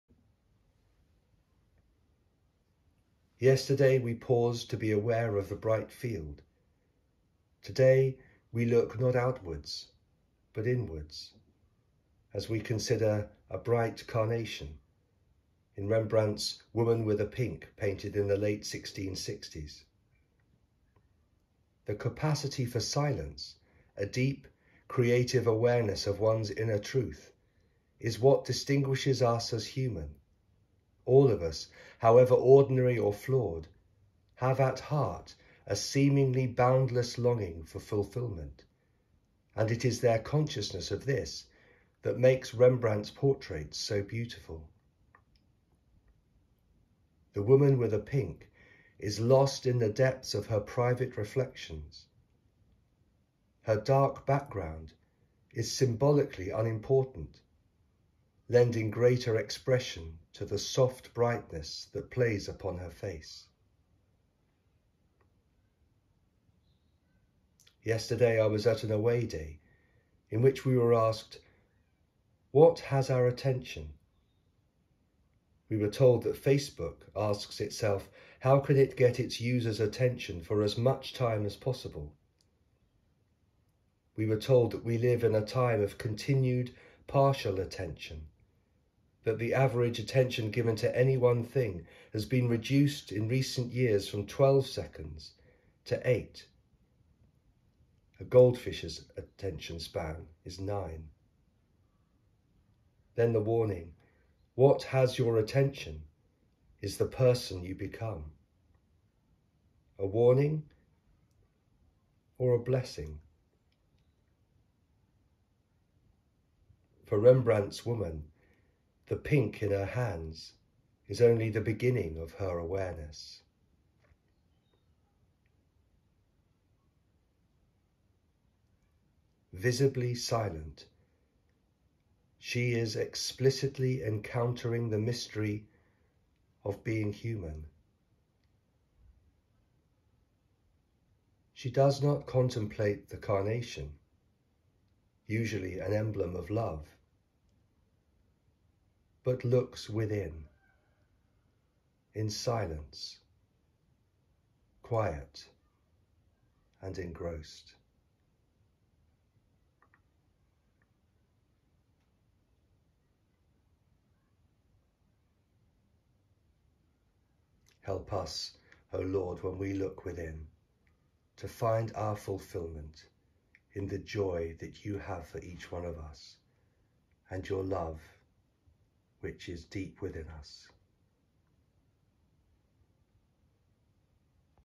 Lent Reflection Tuesday 8 March 2022: 'The woman with the pink' by Rembrandt (c1666)
Reflection based on words by Sister Wendy Becket. 'The Art of Lent'